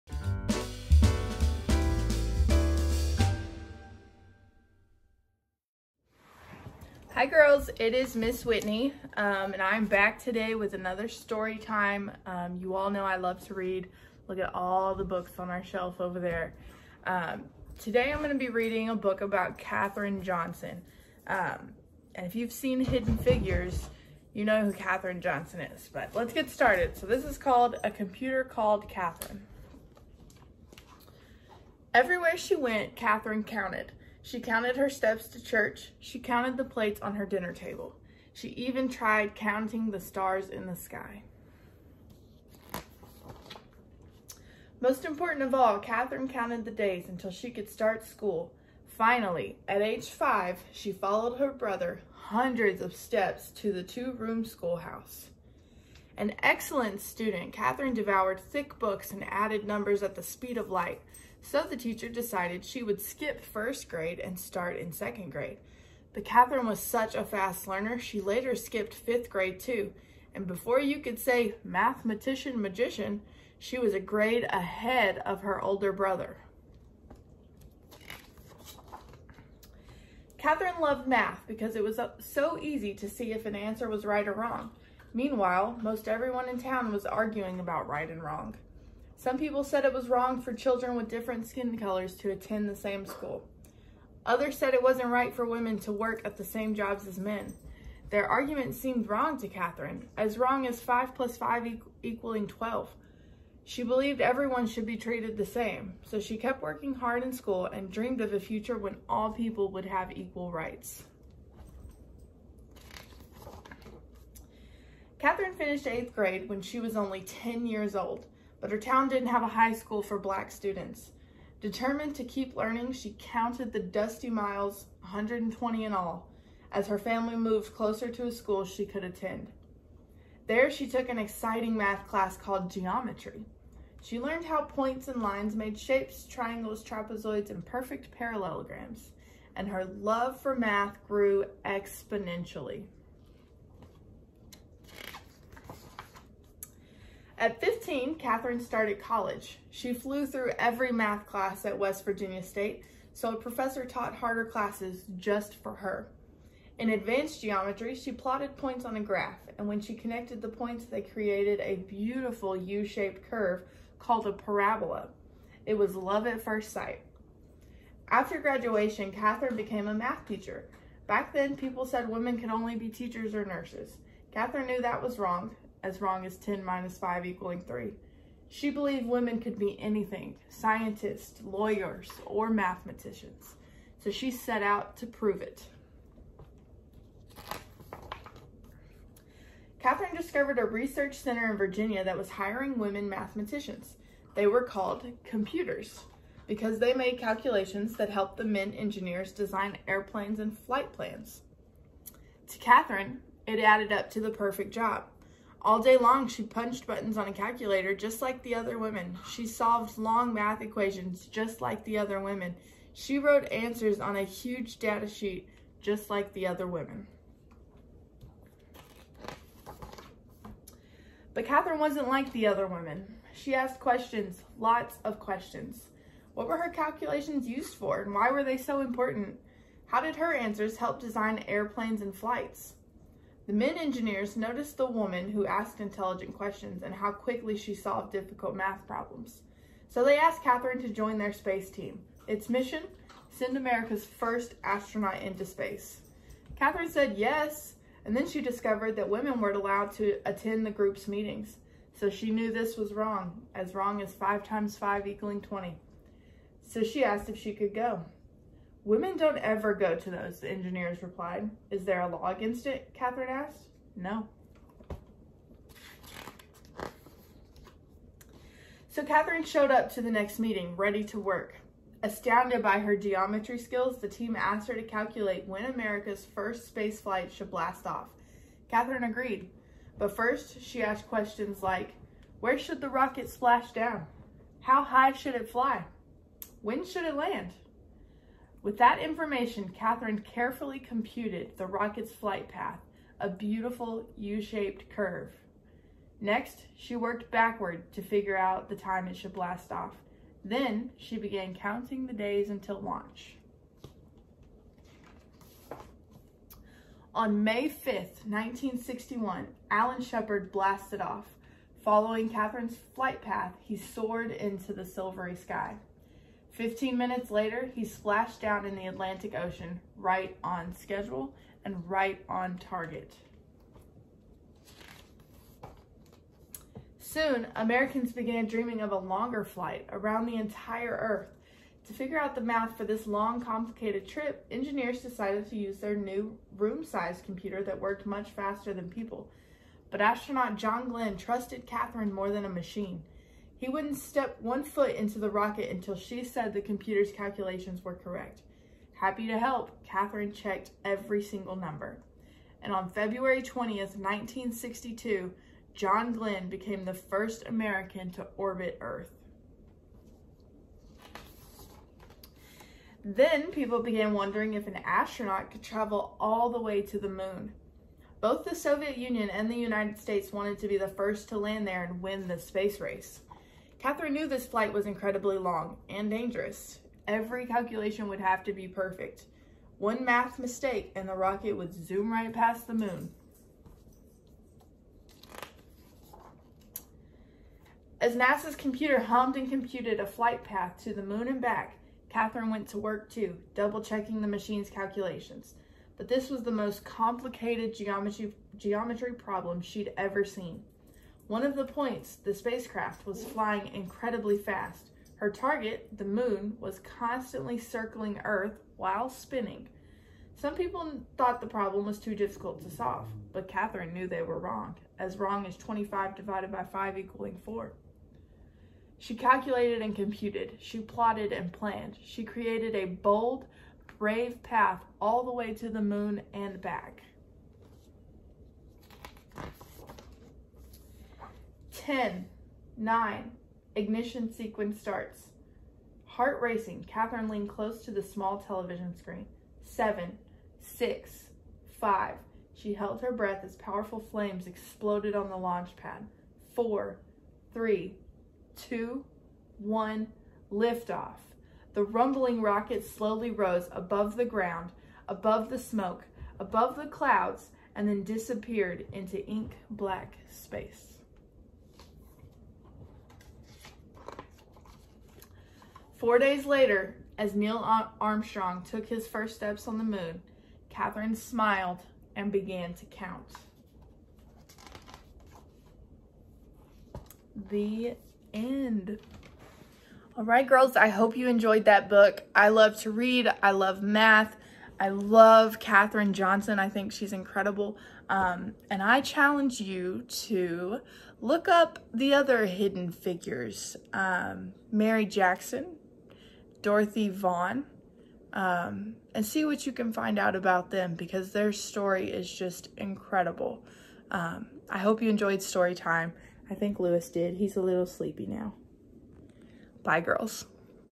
Story Time with Ms. Whitney!
Hi girls! Happy September!

Today, Ms. Whitney is here to do STORYTIME! Today’s book is about the AMAZING, STRONG, SMART, and BOLD Katherine Johnson!

Listen along. Afterwards, I challenge you to do your own research on all the “Hidden Figures” — Katherine Johnson, Mary Jackson, and Dorothy Vaughan.